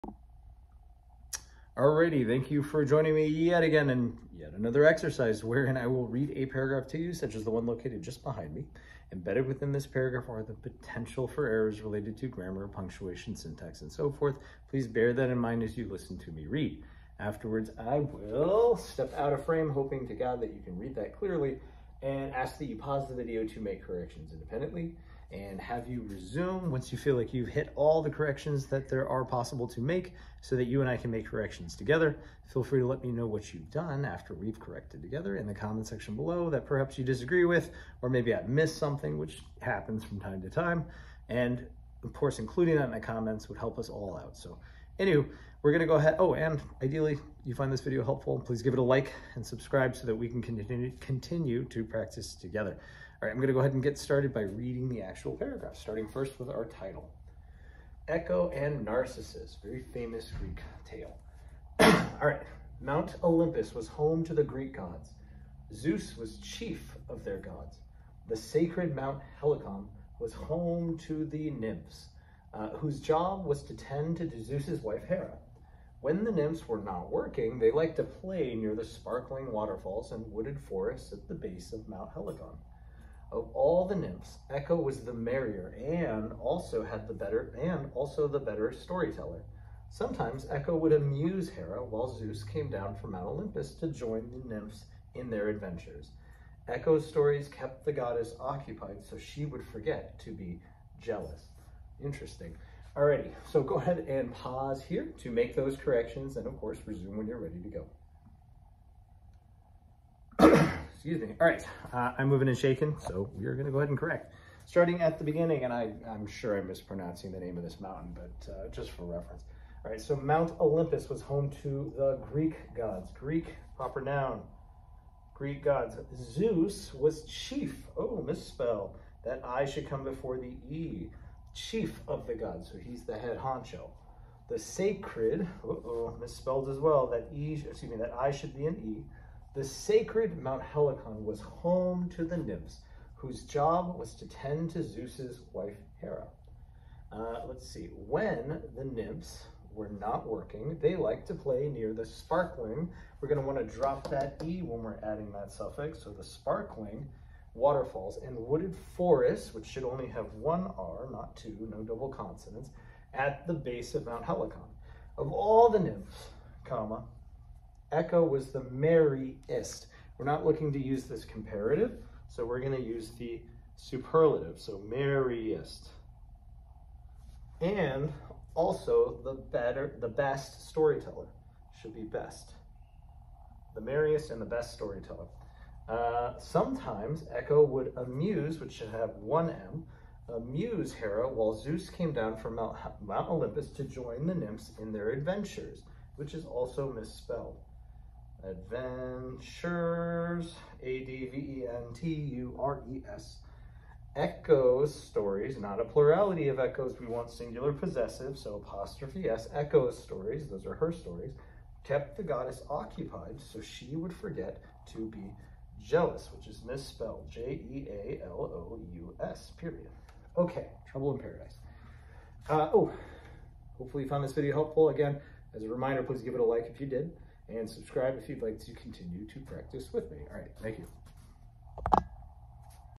English Paragraph Correction/Editing: Advanced, Exercise Thirteen
Join me as we engage in a proofreading-exercise, designed to aid in your English practice and learning.                               @LearnEnglishWithMatta                                   ​